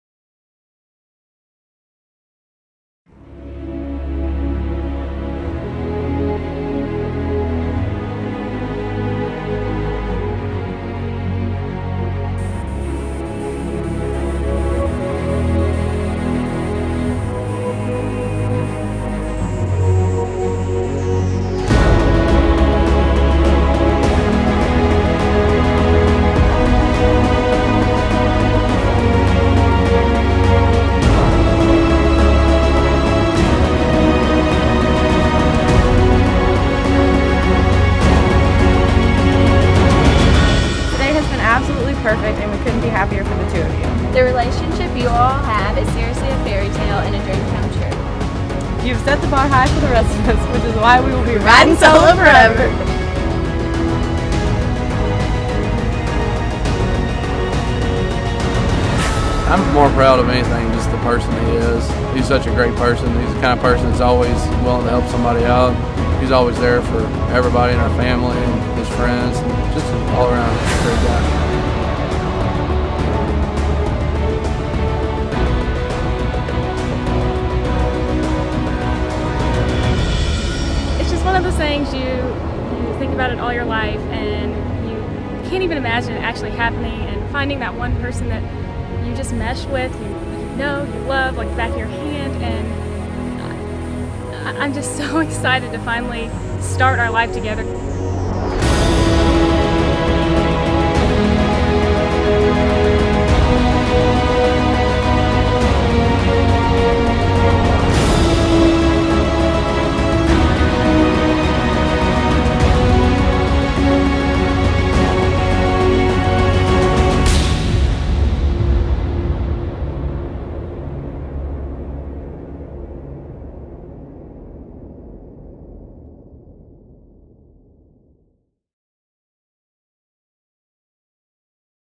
Grieshaber Wedding Trailer
Hannah and Mark have been together but today began the first day of the rest of their lives!